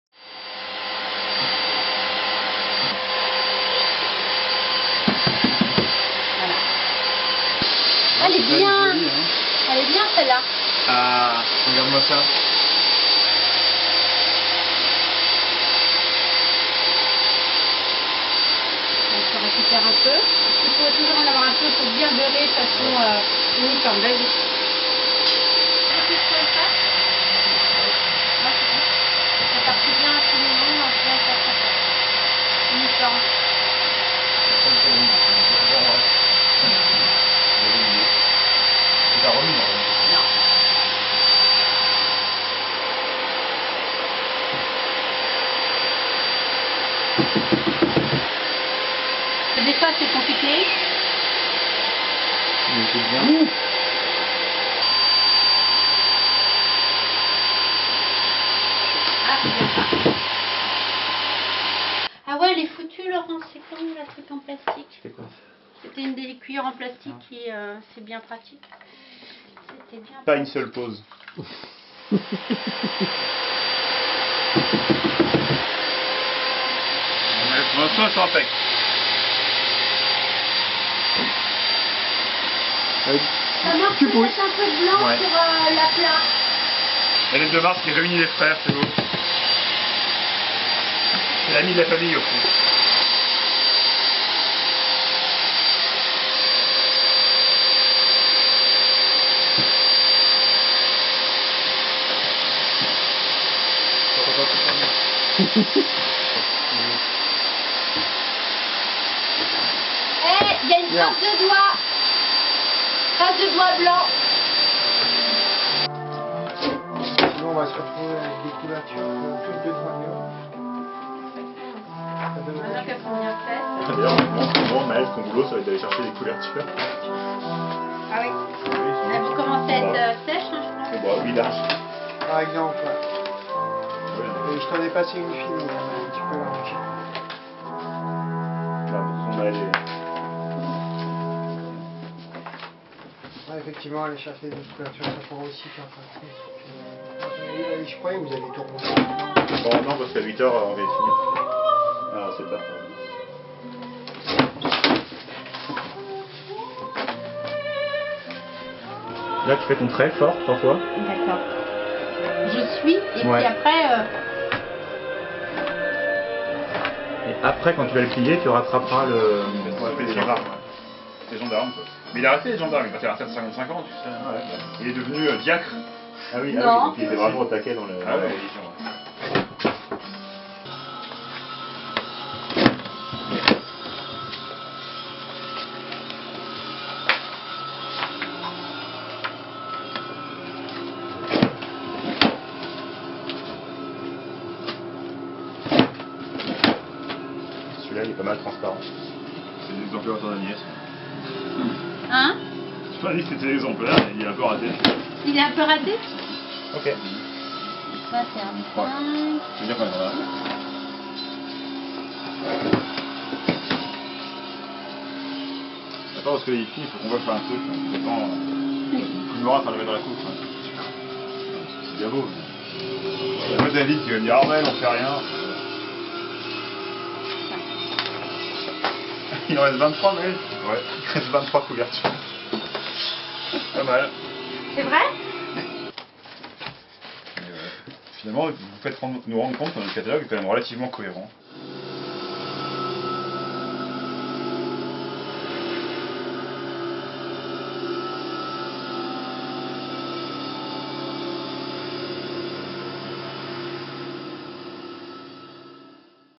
Un vent violent - Un livre de C. de Trogoff aux éditions L'Égouttoir
Aux ateliers de la PCCBA, avec Maël & Gwendal Rannou, C. de Trogoff & L.L. de Mars, réalisation de la couverture de "Un vent violent" en sérigraphie.